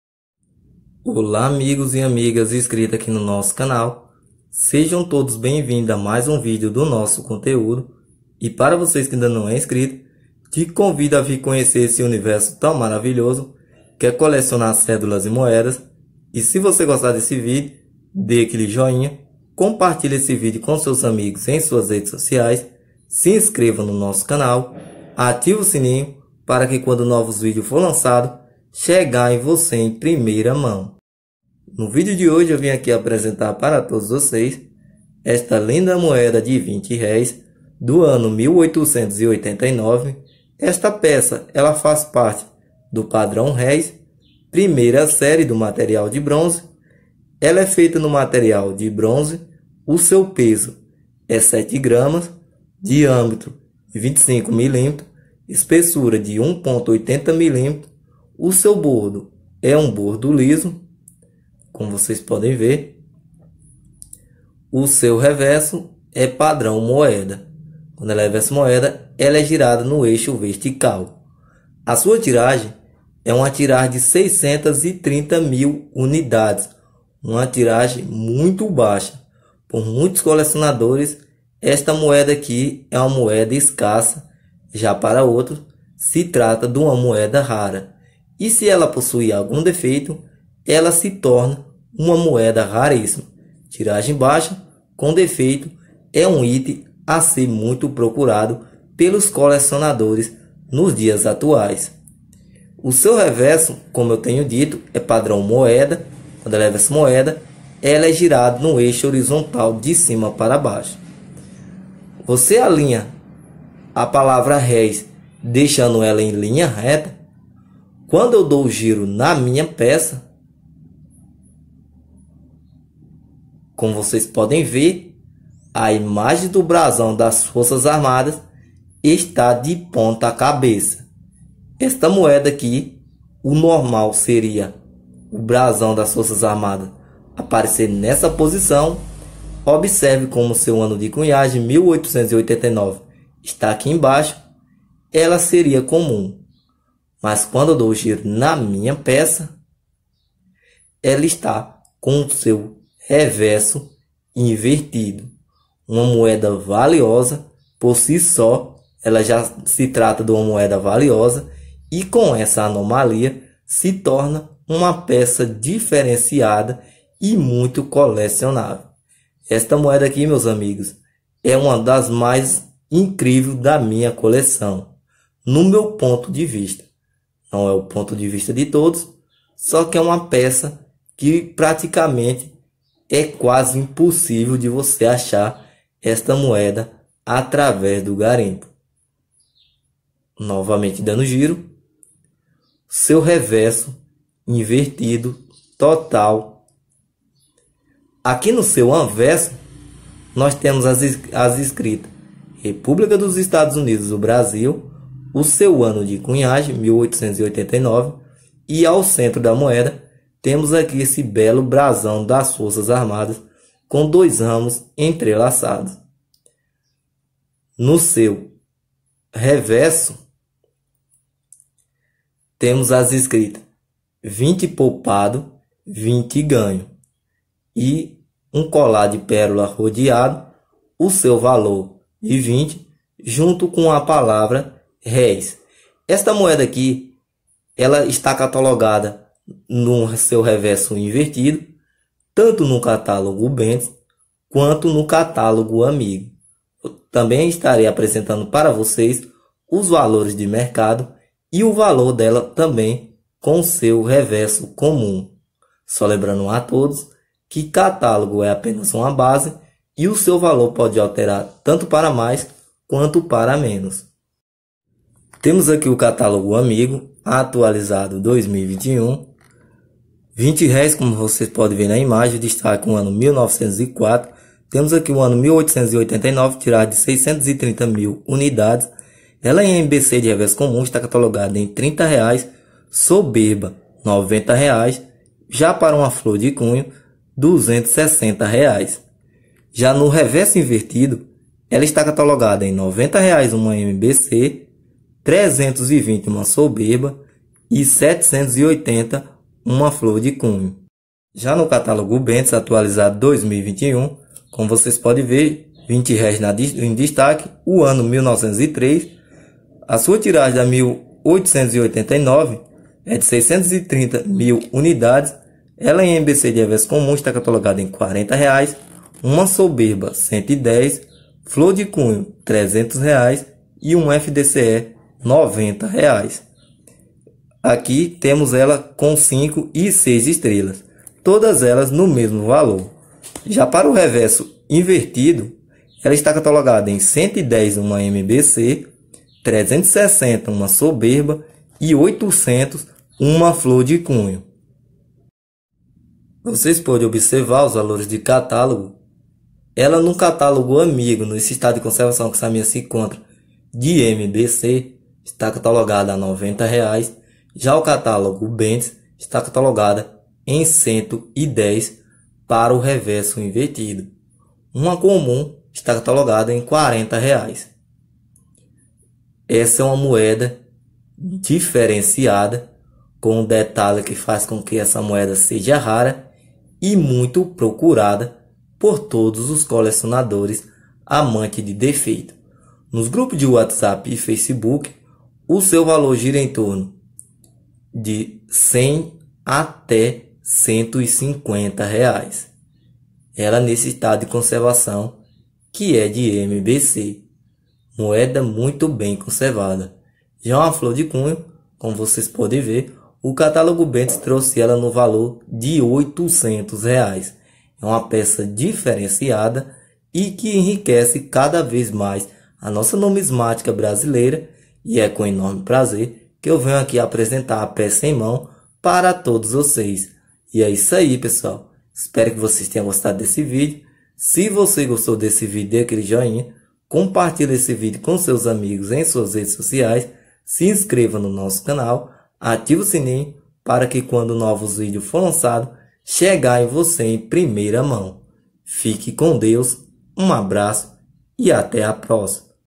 rara e diferenciada 20 réis 1889. valiosa com defeito cobiçado por muitos colecionadores, moeda top
olá amigos e amigas seja todos bem vindo ao nosso canal NUMISMATICA HBP COLEÇÃO, no video de hoje estarei apresentando a moeda de 20 reis com defeito do reverso invertido valioso e diferenciado
plano réis: 1 serie bronze
material: bronze
peso: 7,00 gr
diametro: 25,00 mm
espessura: 1,80 mm
bordo: liso
reverso: moeda
tiragem: 630.000 und
origem: Brasil
produzido: casa da moeda Rio de janeiro